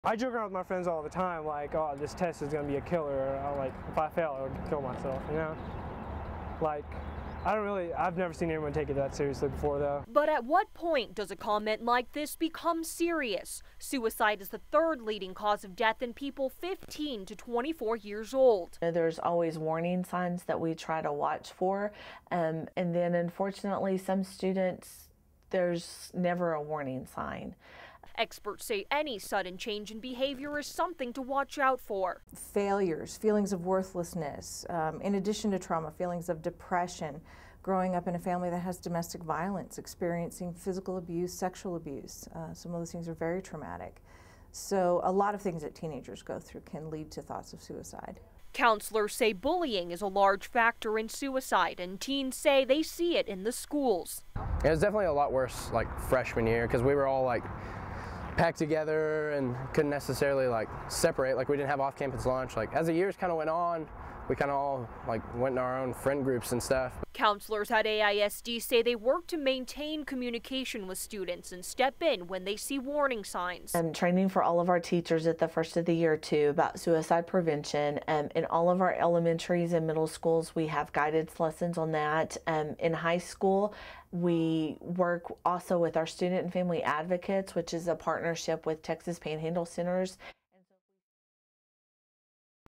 Teen and young adult suicides affect thousands every year
More than 4,600 15 to 24-year-olds commit suicide every year, according to the Centers for Disease Control